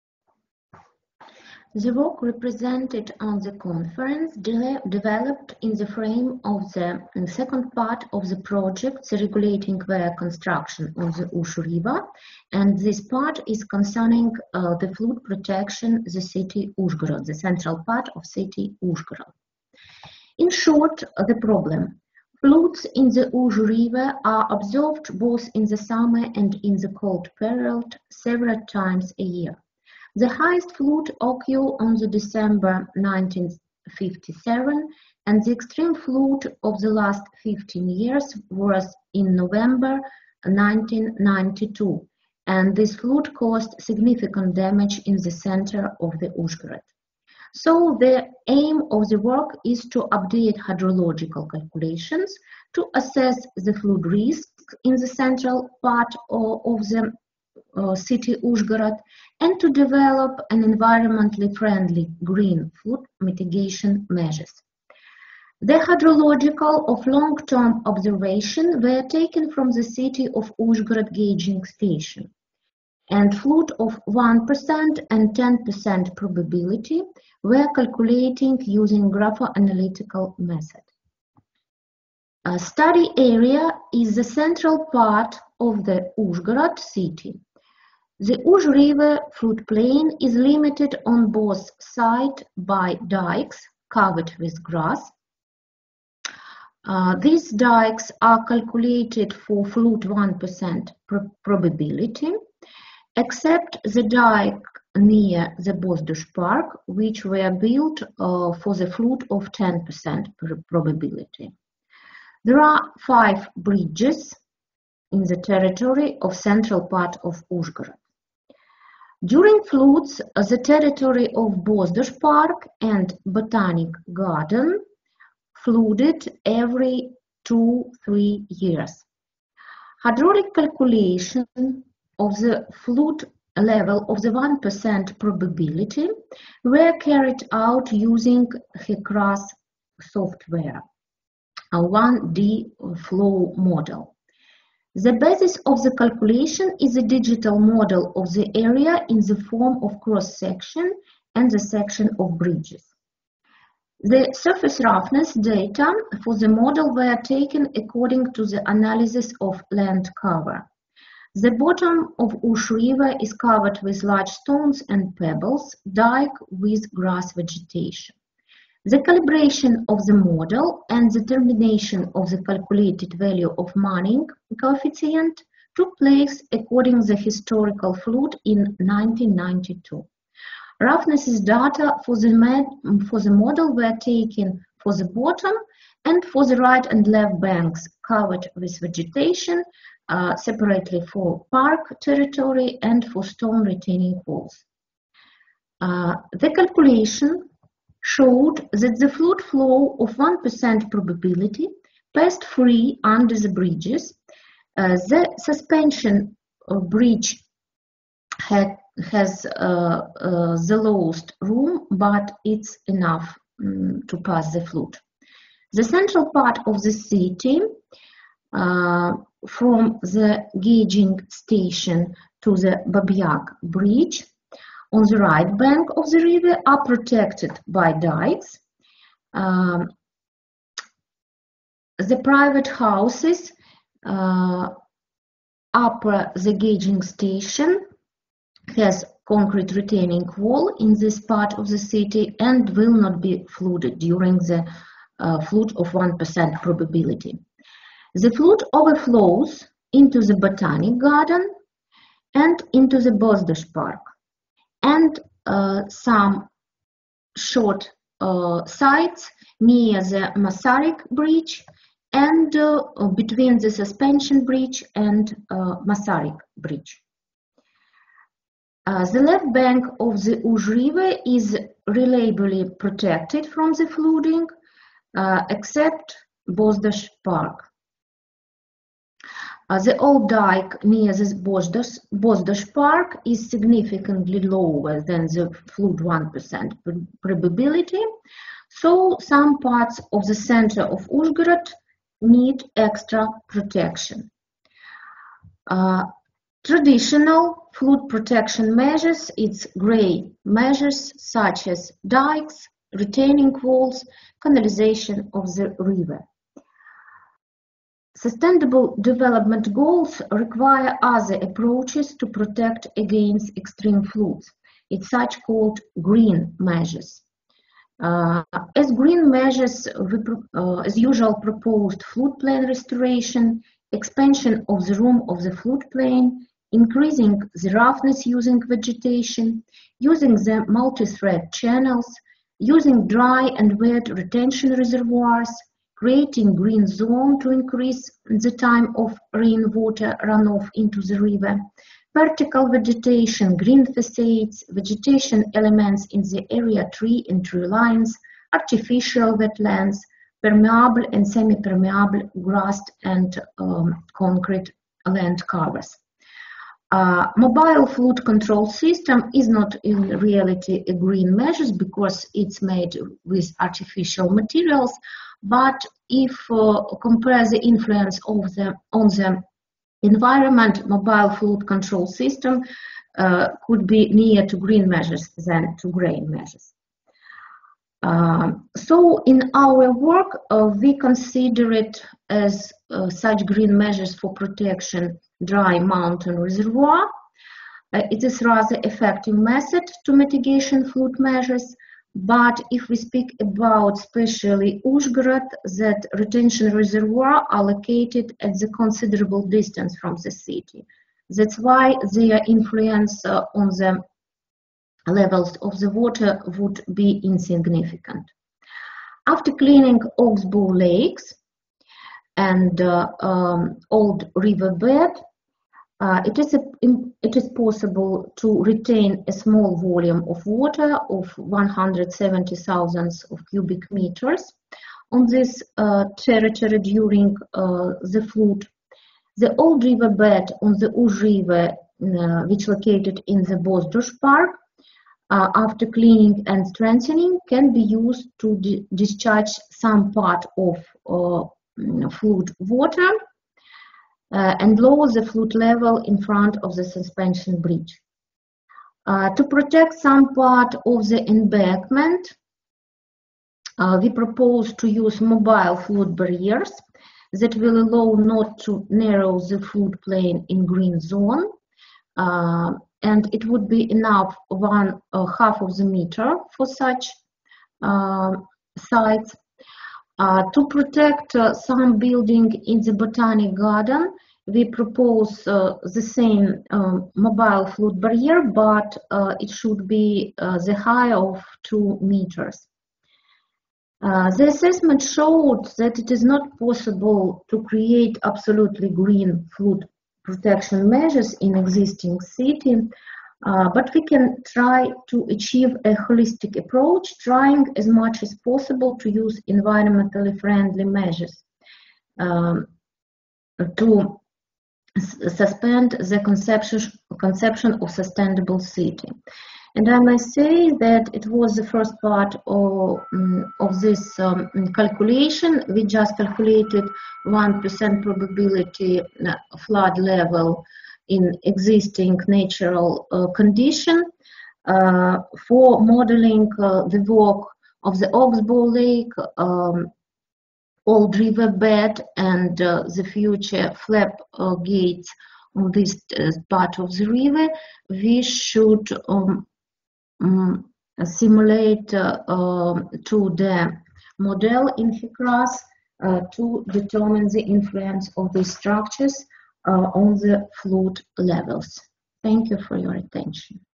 ICSF2024. Evaluation of the green flood mitigation measures in urban area in the frame of the sustai
Evaluation of the green flood mitigation measures in urban area in the frame of the sustainable city concept
Velychko Svitlana, Olena Dupliak